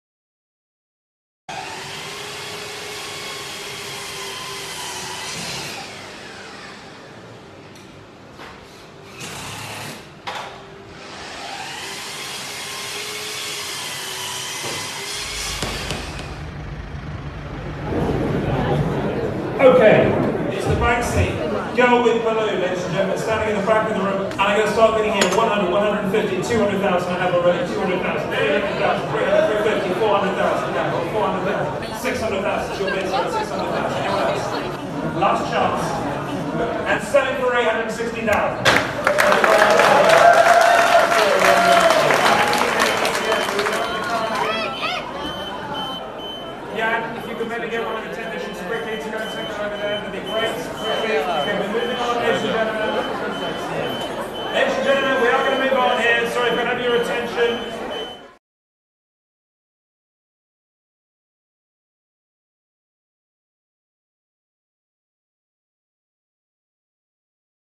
BANKSY explica cómo montó LA TRITURADORA en el cuadro
El artista revela que montó las cuchillas en el cuadro hace unos años por si salía a la venta.